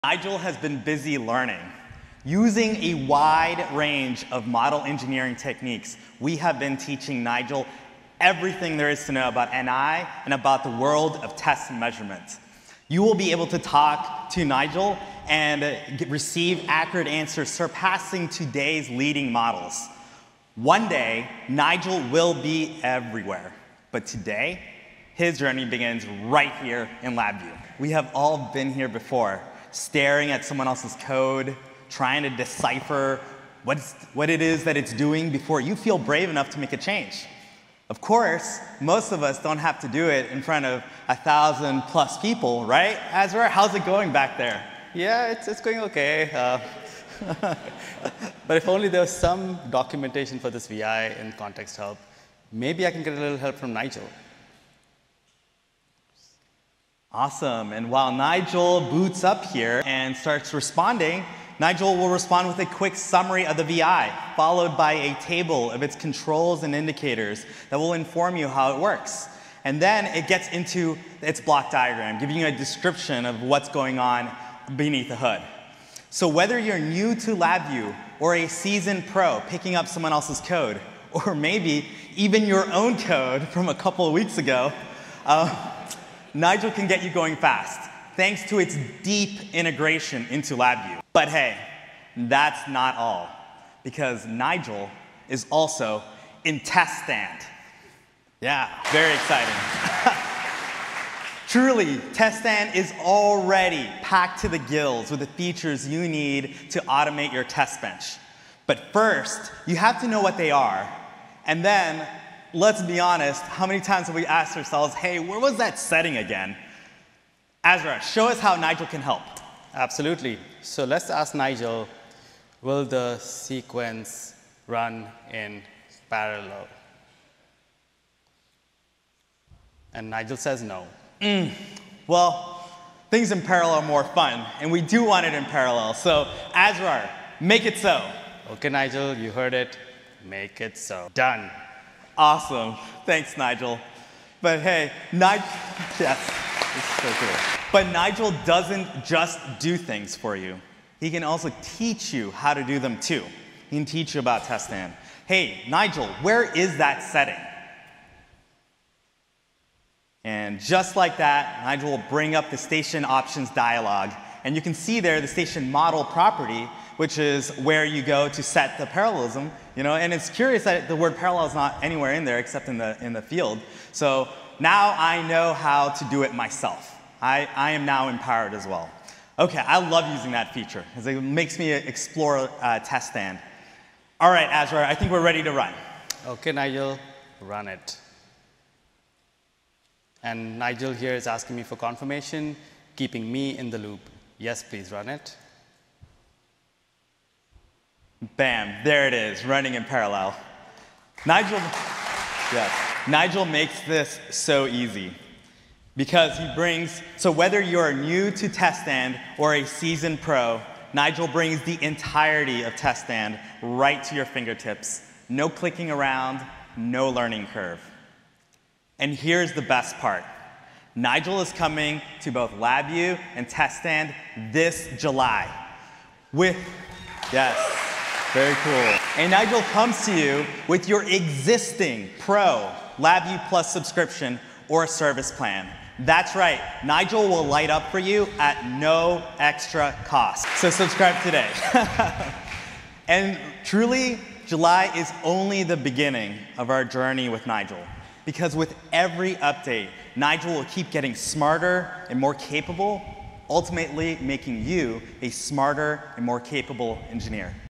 See NI Nigel™ AI Advisor in action with NI LabVIEW and TestStand
Experience the power of Nigel™ AI Advisor in NI LabVIEW and TestStand! Watch this video to see how Nigel explains VIs, automates test benches, and helps you learn as you go—making engineering faster and smarter. Launching this July with your LabVIEW+Suite subscription at no extra cost.

Dive deeper into the world of Nigel™ by tuning in to this session